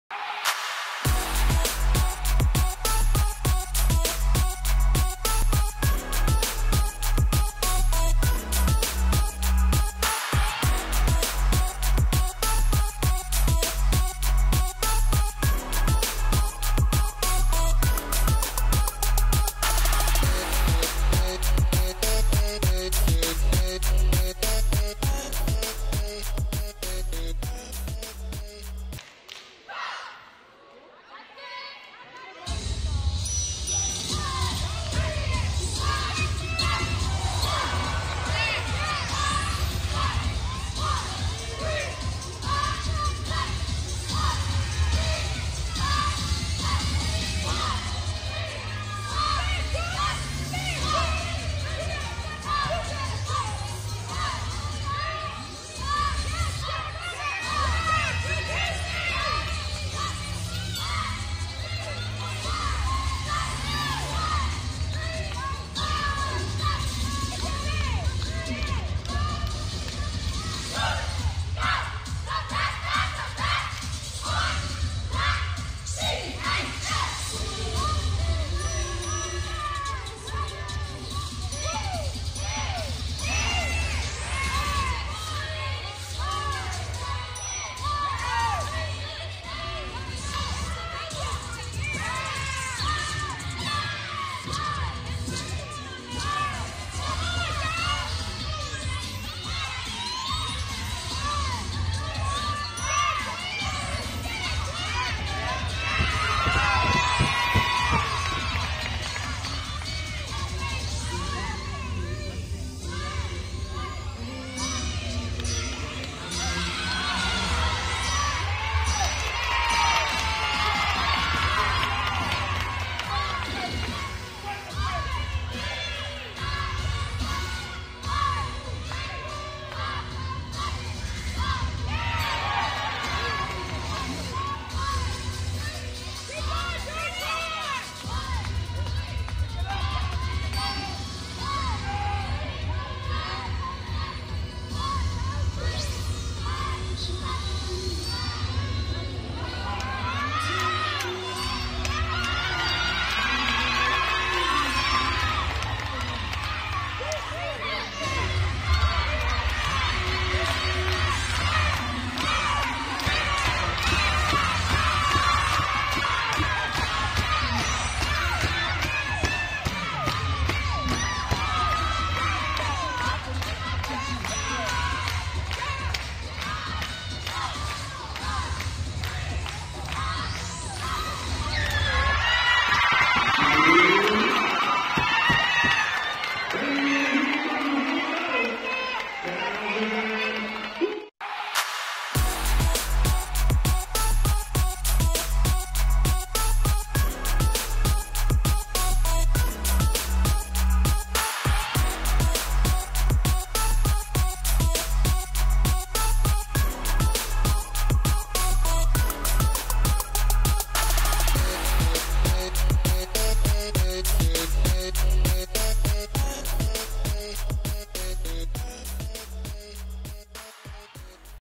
Coolidge Colts DCIAA Cheerleader Championship Performance
The Scholar Athletes Sports Network SASN.TV was at the DCIAA 2022 Cheerleading Championship at Coolidge High School in Washington DC,  Coolidge Colts Cheerleading team performance for the 2022 season in the DMV.